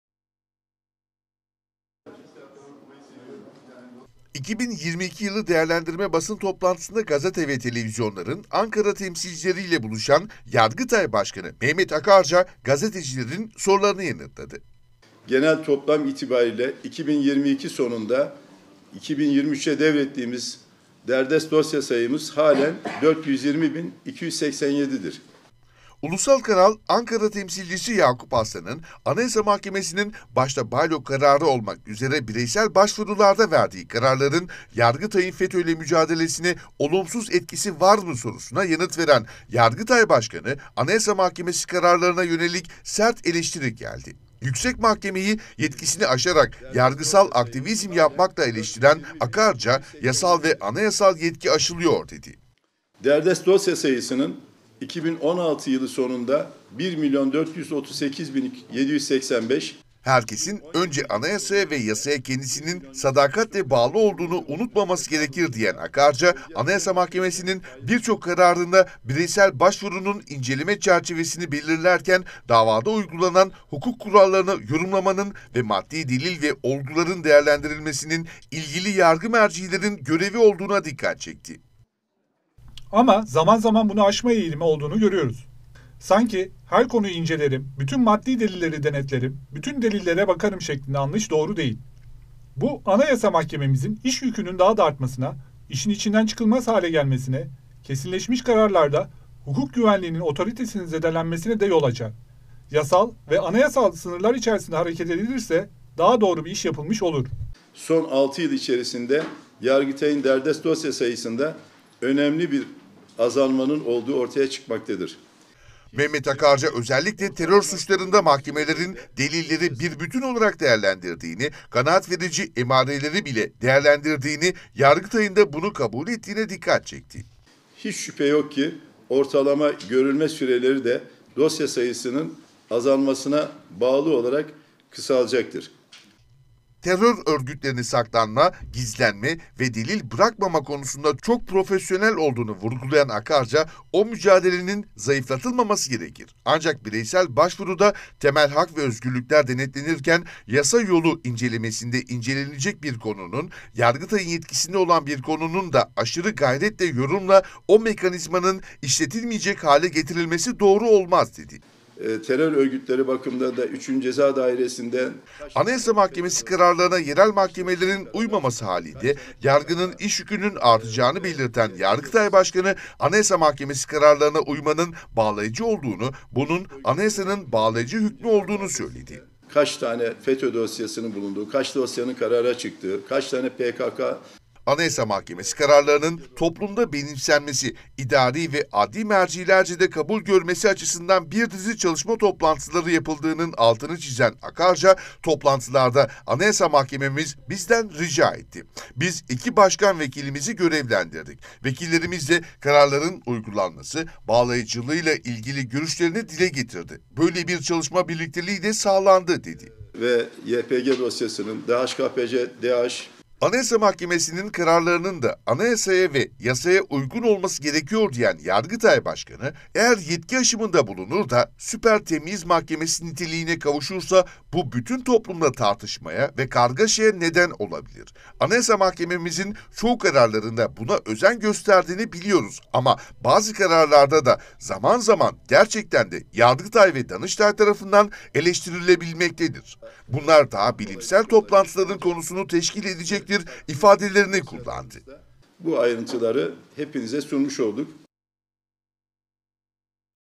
Yargıtay Başkanı'ndan AYM'ye eleştiri: "Yasal ve Anayasal yetkisini aşıyor"
Yargıtay Başkanı Mehmet Akarca Anayasa Mahkemesini yargısal aktivizm yapmakla eleştirdi. Ulusal Kanal'ın, Anayasa Mahkemesinin  başta Bylock kararı  olmak üzere bireysel başvurularda verdiği kararların Yargıtay'ın Fetö ile mücadelesine olumsuz etkisi var mı? sorusuna yanıt veren Yargıtay başkanı, AYM bazı bireysel başvurularda ''Yasal ve anayasal yetki aşılıyor'' dedi